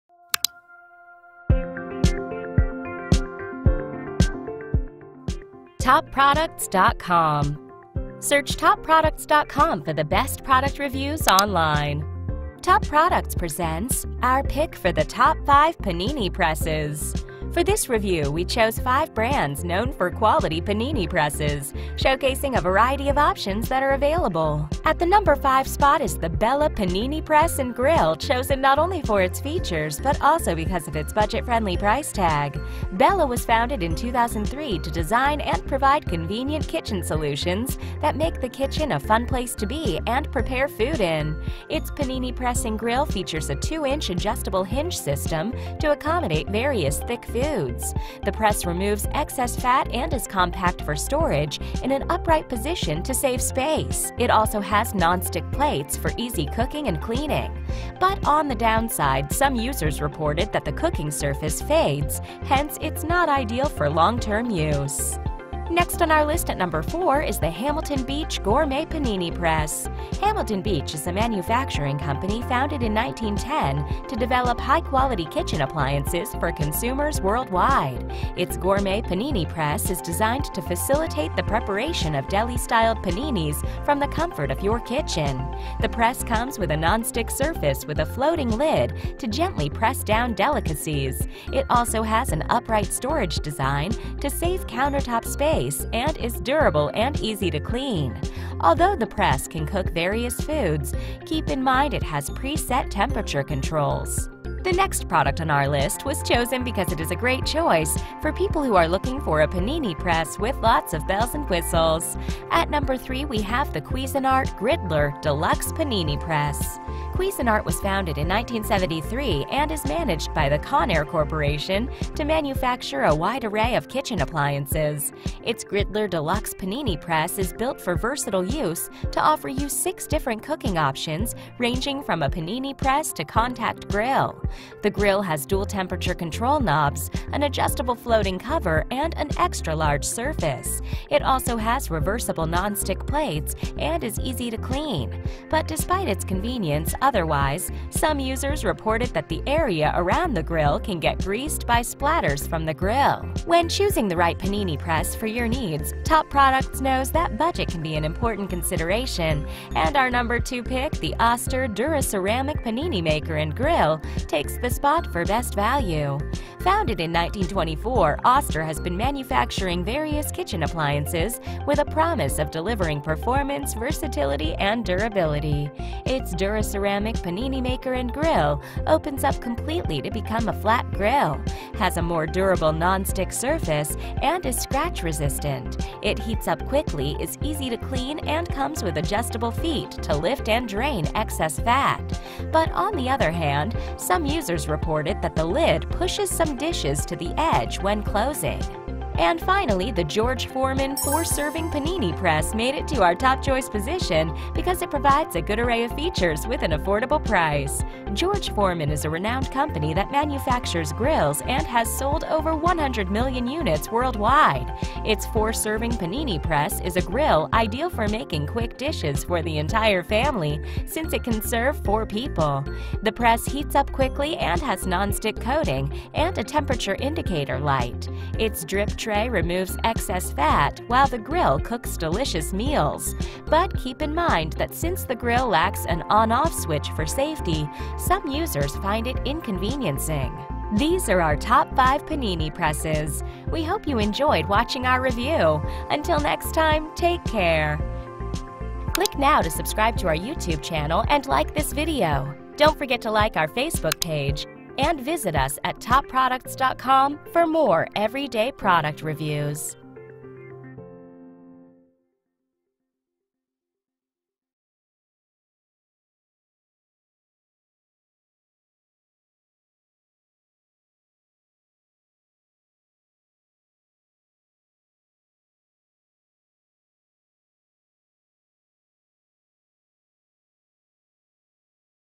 Best Panini Press Reviews – How to Choose the Best Panini Press
Our trained experts have spent hours researching the best Panini Presses available on the market. In order to save you both time and money, we’ve narrowed the field down to some of the best Panini Press brands. Check out an in-depth review on TopProducts.com: Best Panini Press Reviews

Paninis: some of the most delicious-looking (and tasting) sandwiches on the planet. You can make them at home, too, with the help of a simple device called a panini press. What makes panini presses even better? They can make other foods too.

If you’re in the market for a panini press (who shouldn’t be?), then you’re in the right place. Our team has put in some research time to make a list of the top five in the market, to show you the ins and outs of each. You’re sure to find one that’s perfect for your kitchen either in our list, or from one of our featured brands. We hear your stomach growling—you’d better make it snappy!

In this video, you will learn about the features, pros and cons for our picks for the top Panini Presses. Our experts have chosen a wide range of products that can fit every budget, from top-of- the-line to budget friendly. This video includes:

• George Foreman 4-Serving Panini Press Review
• Oster DuraCeramic Panini Maker and Grill Review
• Cuisinart Griddler Deluxe Panini Press Review
• Hamilton Beach Gourmet Panini Press Review
• Bella Panini Press and Grill Review